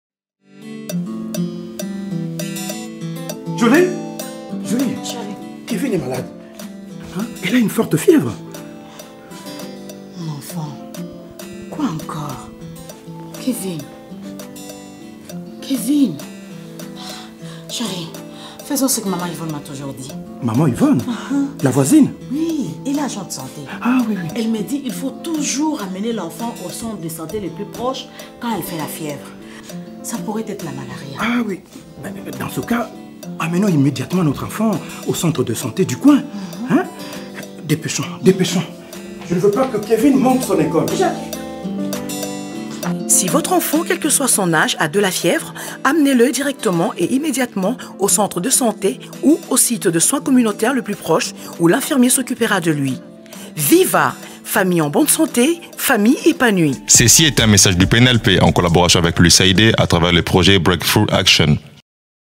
Breakthrough ACTION-DRC VIVA! Classic Campaign Spots: Malaria - Prompt Care Seeking
Breakthrough ACTION-DRC collaborated with the Ministry of Health to develop and implement a community-based campaign promoting the adoption of essential household healthy practices and facility-based care seeking. The campaign was guided by a human centered design and the FEAST (fun, easy, attractive, social, timely) conceptual model. The campaign included a coordinated package of interventions comprised of social events, competitions, savings schemes, community engagement, and mass media to deliver SBC messages on healthy priority behaviors related to six health areas. These videos were developed as part of the mass media interventions to complement the VIVA! community interventions.

Breakthrough ACTION-RDC a collaboré avec le ministère de la Santé pour développer et mettre en œuvre une campagne communautaire visant à promouvoir l'adoption de pratiques familiales essentielles et la recherche de soins dans les centres de santé. La campagne a été guidée par une conception centrée sur l'humain et le modèle conceptuel FEAST (amusant, facile, attrayant, social, et opportun). La campagne comprenait un ensemble d'interventions coordonnées comprenant des événements sociaux, des concours, des interventions d'épargne, un engagement communautaire et des médias de masse pour diffuser des messages de CSC sur les comportements de santé prioritaires liés à six domaines de la santé. Ces vidéos ont été développées dans le cadre des interventions dans les médias de masse pour renforcer les interventions communautaires de la campagne VIVA !